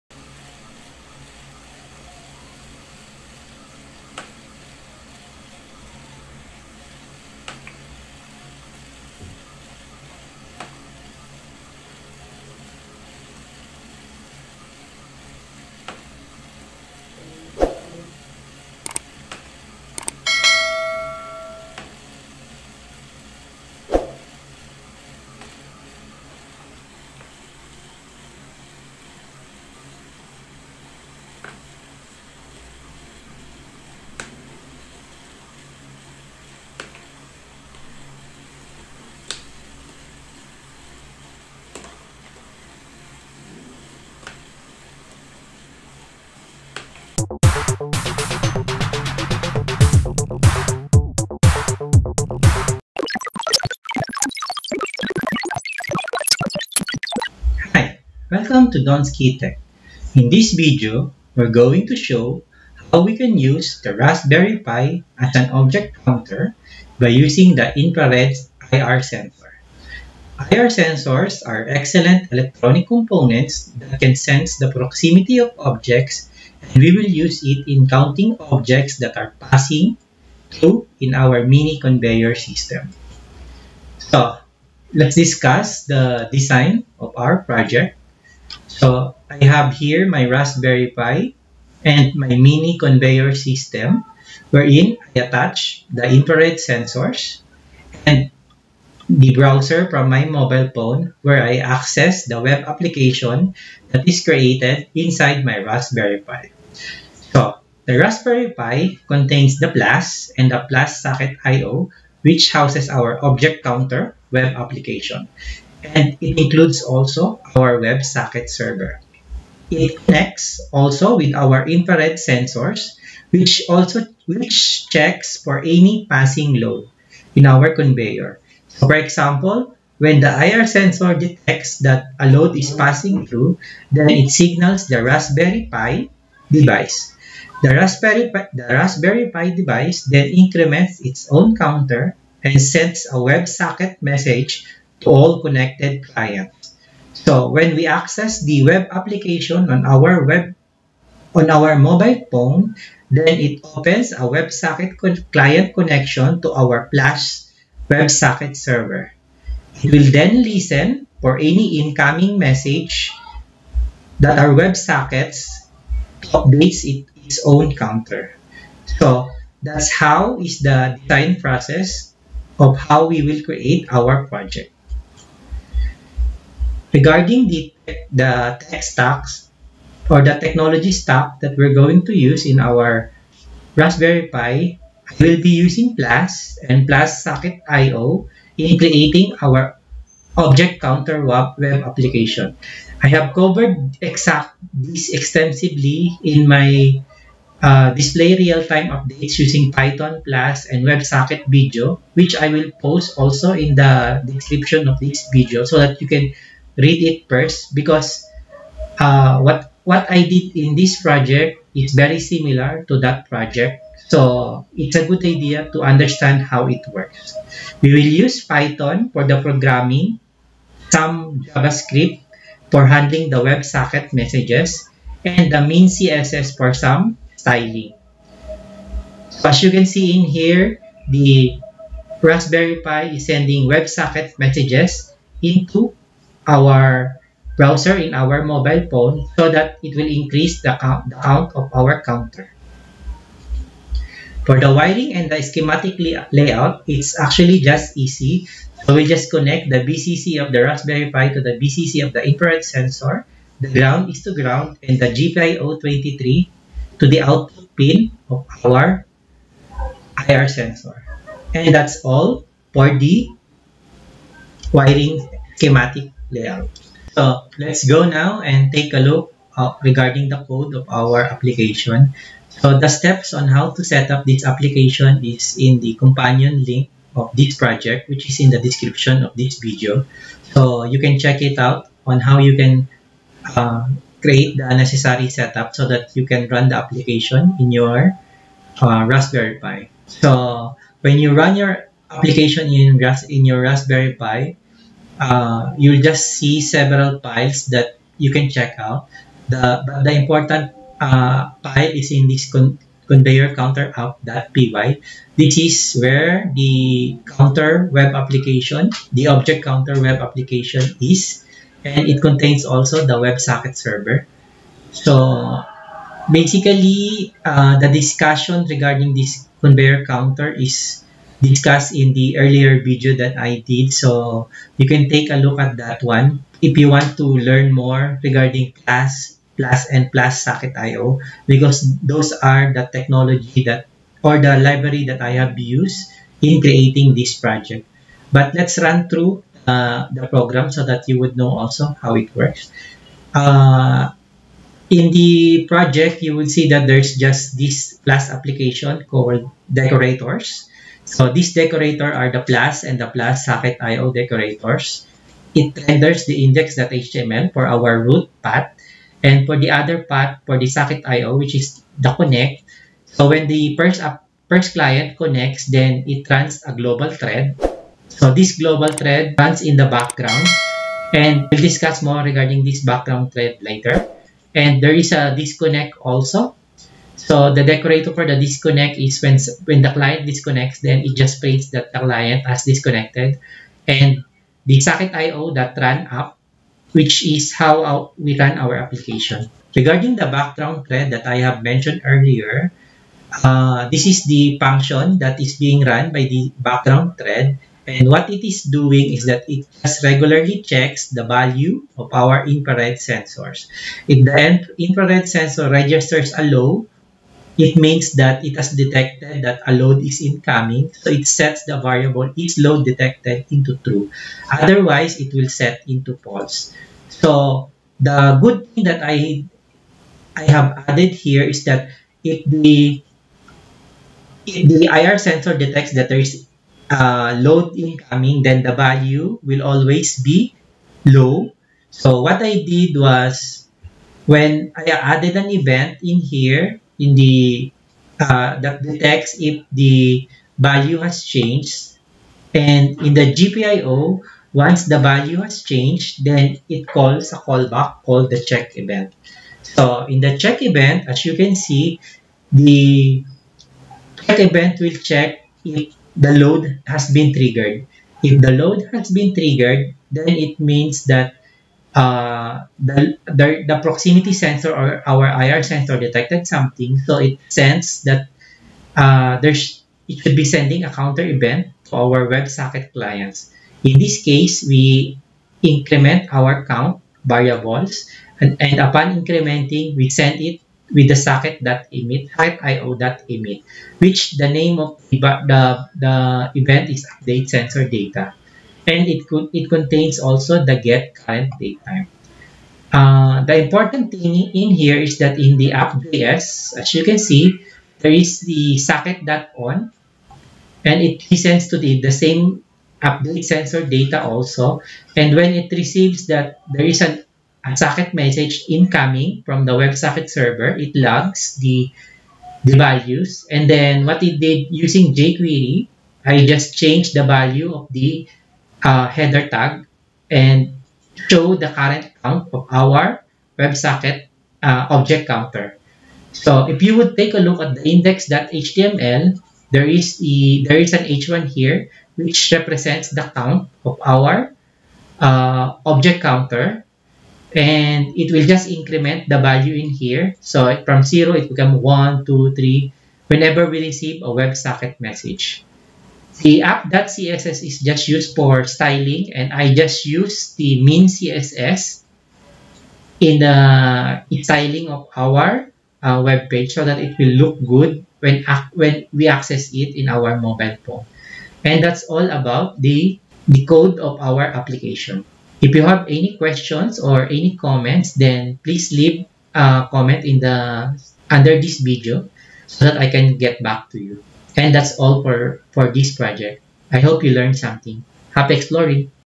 Raspberry Pi Object Counting Using Infrared sensor
The following video will show how we can use the Raspberry Pi as an object counter by using the Infrared (IR) sensor. IR sensors are excellent electronic components that can sense the proximity of objects and we will use it in counting objects that are passing in our mini conveyor system.
We will use the WebSockets protocol in programming our Python Flask application as this provides real-time communication between our clients and server.

Happy Exploring!